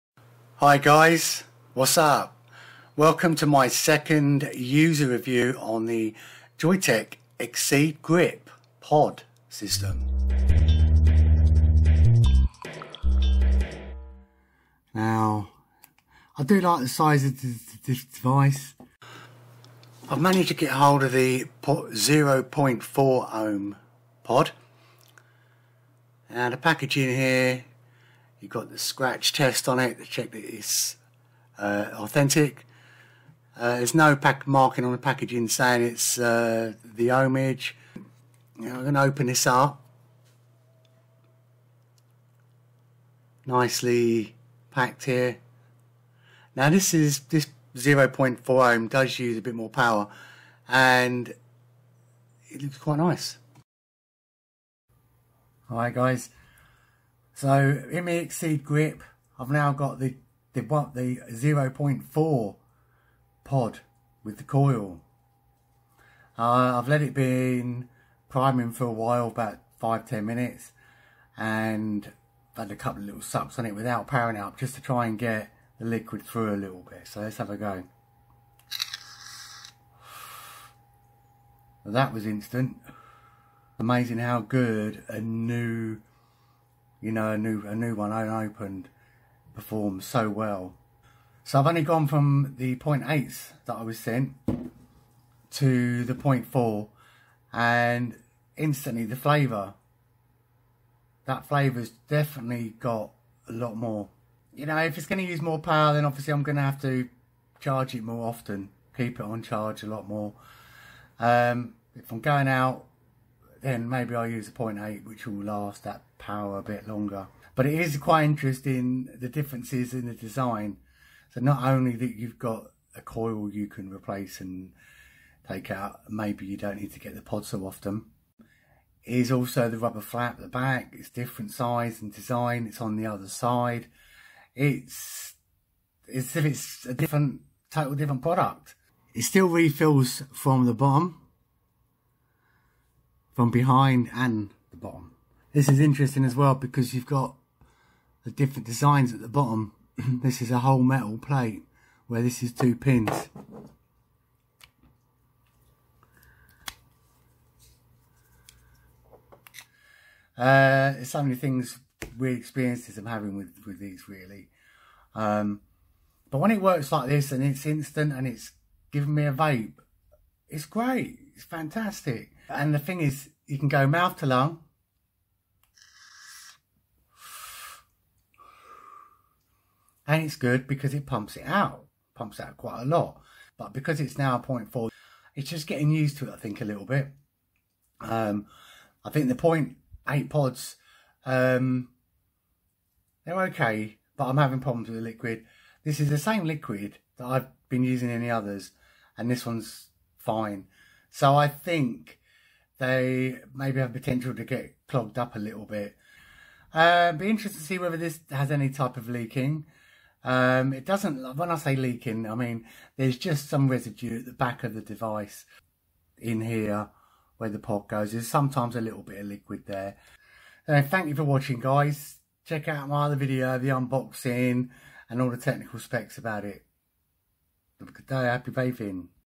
🔞 Exceed Grip Vape Pod Review UK
Update:  if the power button stops, plug it in on charge and try.  This should reset and let it fully charge.

I have both 0.8ohm and 0.4ohm pods in this video.
Changeable EX series coil system. ( Simply unscrew the base and replace with EX head.)

UK Pod system.

Some e-liquids may not vape as good as others    50/50 is recommended.

Joyetech EXCEED GRIP AIO Vape Pod Kit Unboxing 2019